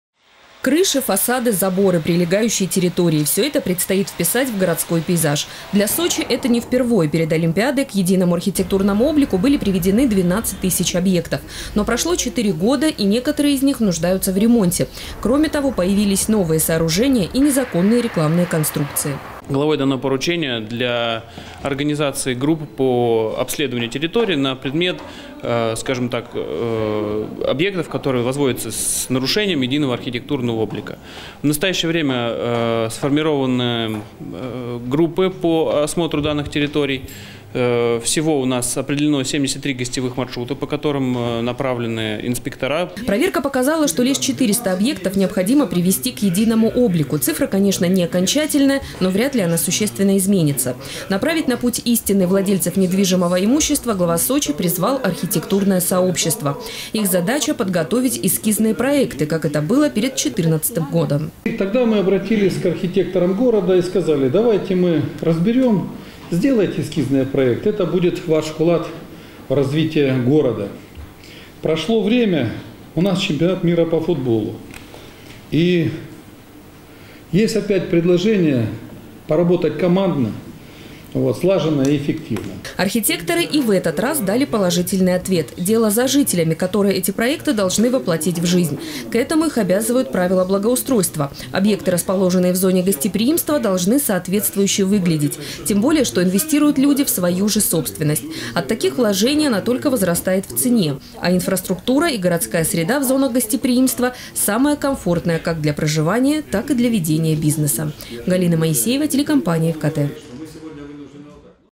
73 гостевых маршрута в Сочи приведут к единому облику. Новости Эфкате
К Чемпионату мира по футболу объекты, входящие в зону гостеприимства, должны быть приведены к единому архитектурному облику. Основная часть работы — позади. Она была проведена перед Олимпийскими играми. Сейчас задача — приструнить тех, кто выбивается из городского пейзажа. Об этом говорили на совещании при главе курорта.

Крыши, фасады, заборы, прилегающие территории — все это предстоит вписать в городской пейзаж. Для Сочи это не впервой. Перед Олимпиадой к единому архитектурному облику были приведены 12 тысяч объектов. Но прошло 4 года и некоторые из них нуждаются в ремонте. Кроме того, появились новые сооружения и незаконные рекламные конструкции.

Сочи. Новости.
Телеканал Эфкате.